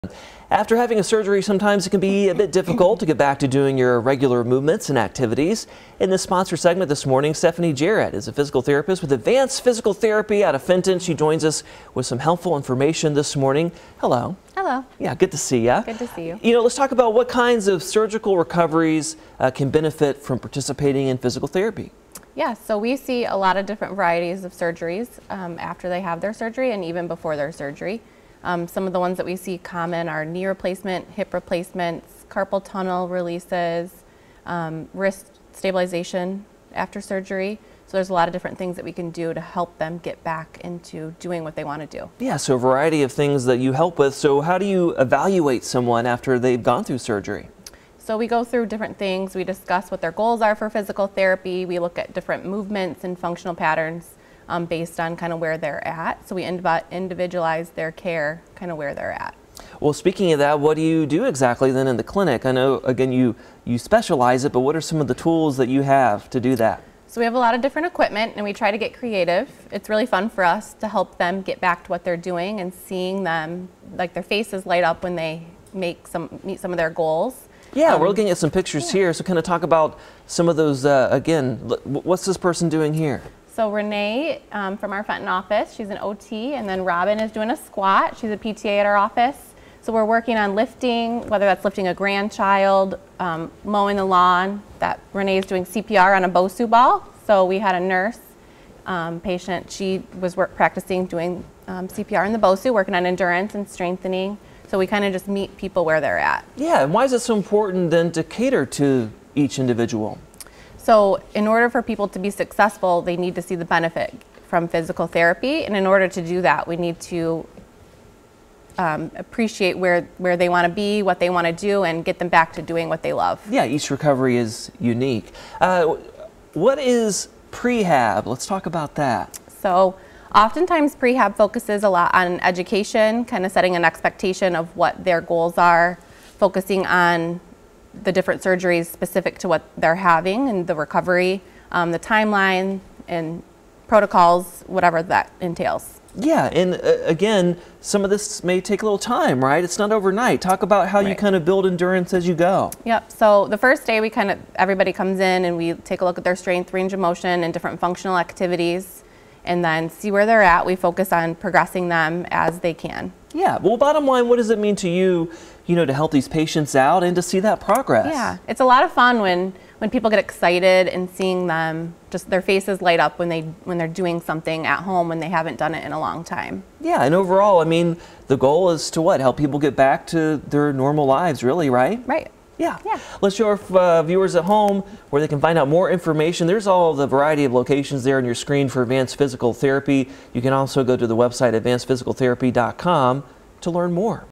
Advanced Physical Therapy offers post-surgery tips
APT offers support to help patients regain movement following surgeries.